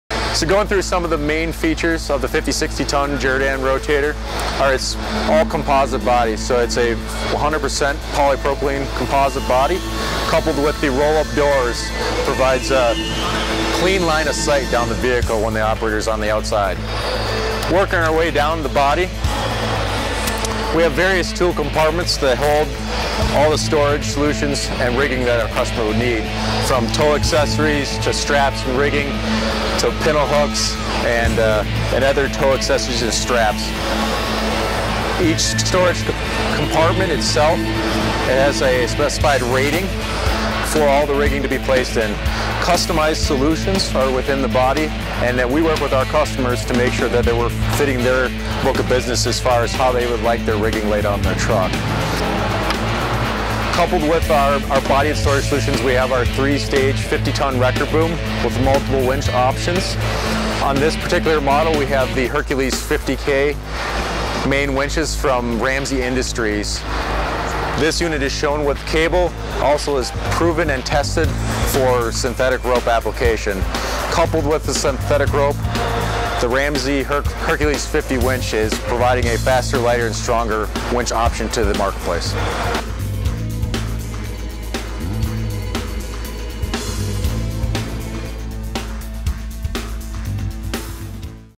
Jerr-Dan's 50/60 rotator and Ramsey Hercules Winch
Jake Perron, chief engineer for Jerr-Dan, does a quick walk thru of Jerr-Dan’s 50/60 rotator and briefly outlines the benefits of the Ramsey Winch Hercules 50k